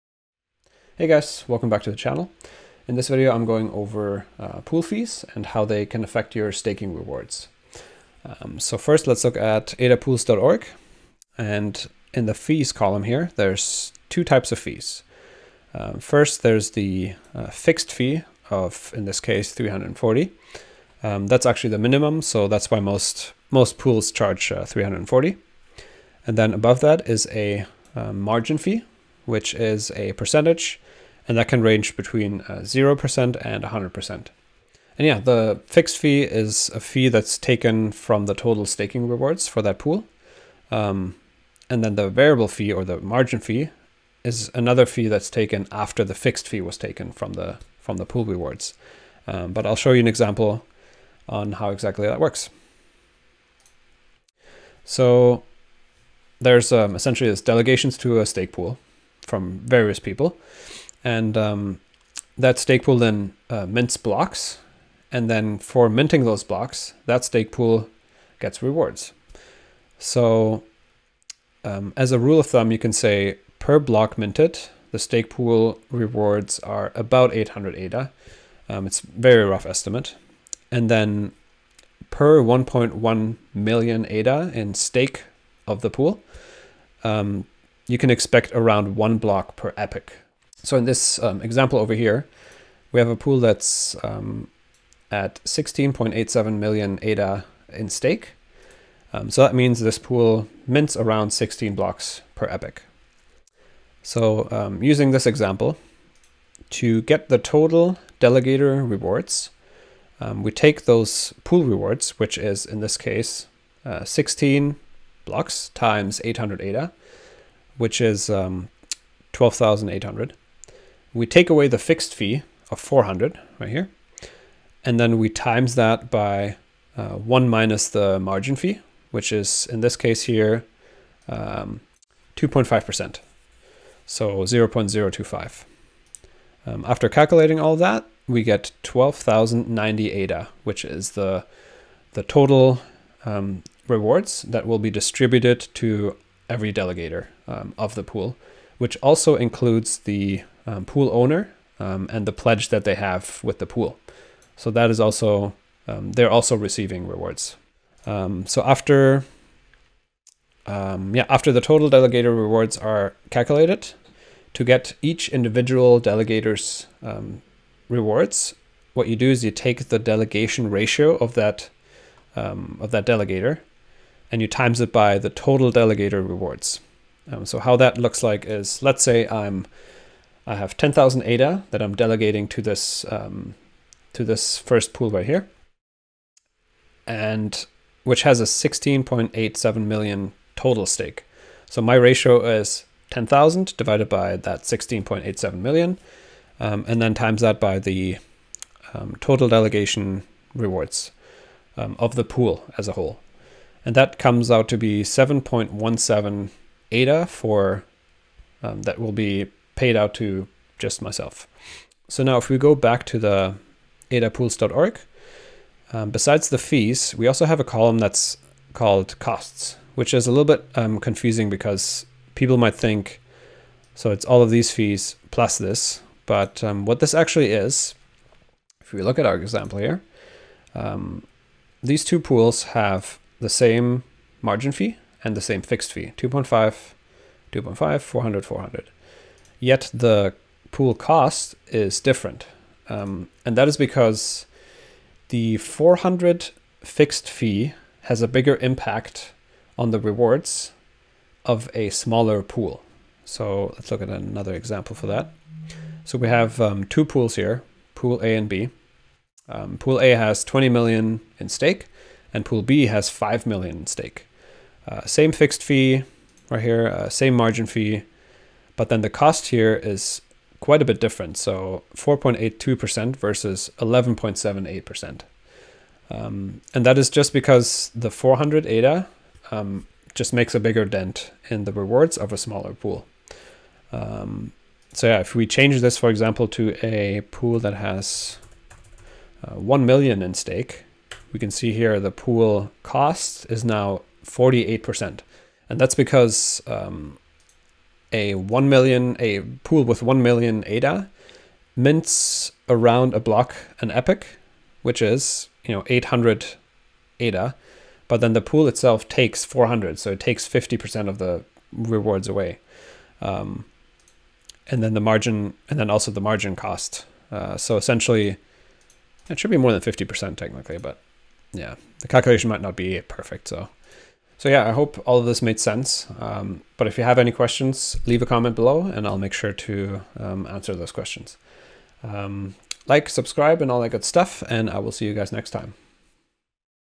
Pool Fees & Staking Rewards Calculation
ℹ️  In this video I am going over how Cardano stake pool fees are structured and how rewards are calculated based on those pool fees.

🔗 Socials

⏱️  Timestamps:
00:00 - Intro
00:08 - Types of Fees
00:49 - Example Calculation
01:44 - Example Calculation - Total Delegator Rewards
02:44 - Example Calculation - Individual Delegator's Rewards
03:31 - Pool Costs
04:14 - Pool Costs - Example
05:24 - Outro